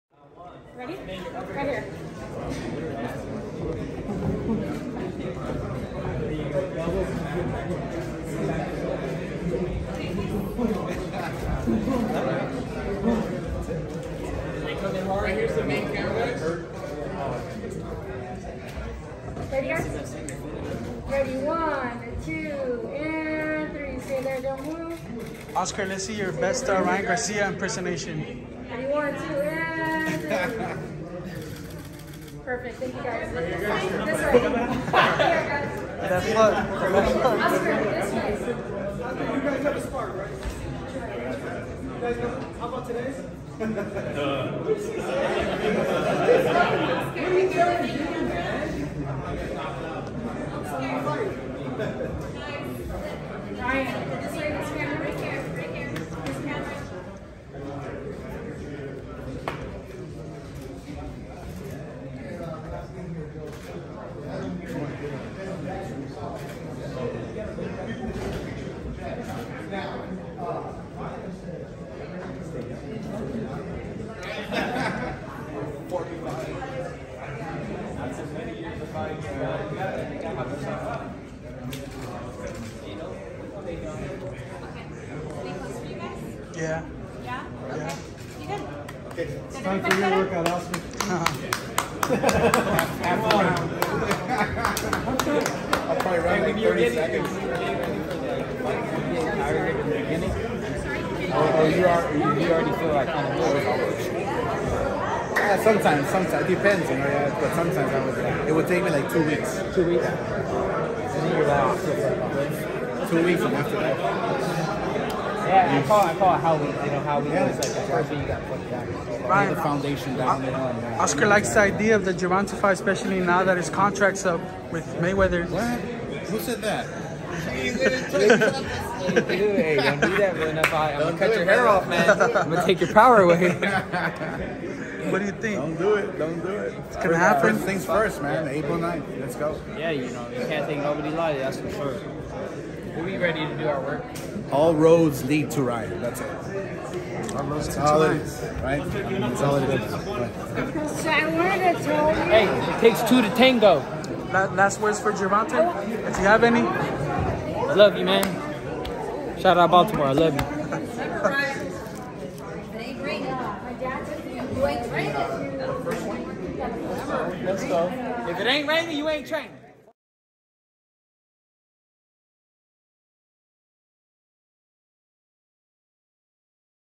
RYAN GARCIA & OSCAR DE LA HOYA SEND GERVONTA DAVIS FIGHT MESSAGE & LAUGH AT MAYWEATHER "CONTRACT UP"
Undefeated lightweight sensation Ryan Garcia hosted a media workout at his gym in San Diego ahead of his return to the ring on Saturday, April 9th, against Emmanuel Tagoe. You don't want to miss how he looked and what went down. Check it out!